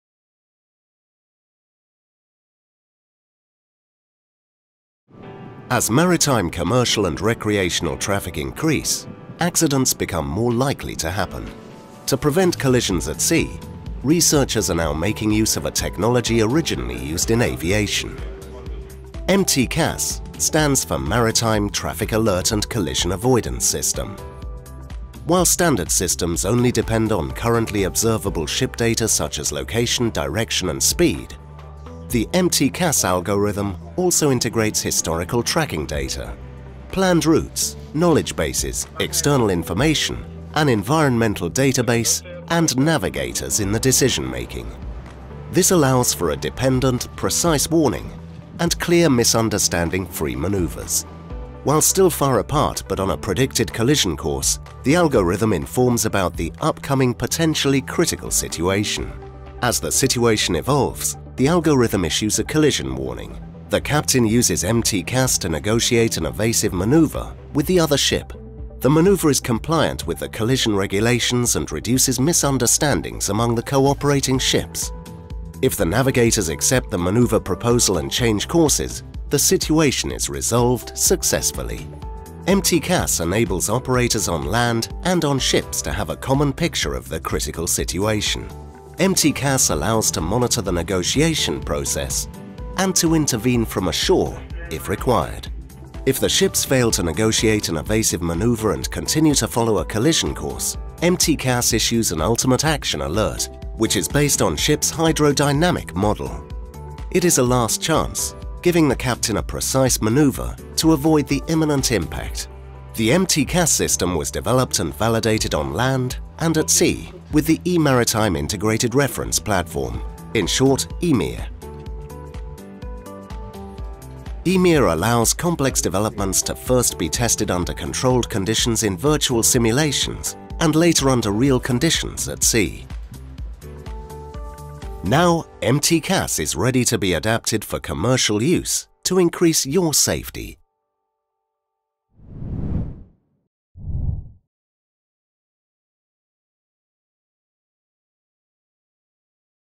Maritime Traffic Alert and Collision Avoidance System MTCAS
As maritime commercial and recreational traffic increase, accidents become more likely to happen.
To prevent collisions at sea, researchers of the MTCAS project are now making use of a technology originally used in aviation. MTCAS stands for Maritime Traffic Alert and Collision Avoidance System.

While standard systems only depend on currently observable ship data such as location, direction and speed, the MTCAS algorithm also integrates historical tracking data, planned routes, knowledge bases, external information, an environmental database and navigators in the decision making. This allows for a dependent, precise warning and clear, misunderstanding free manoeuvres.

Want to learn more about the project?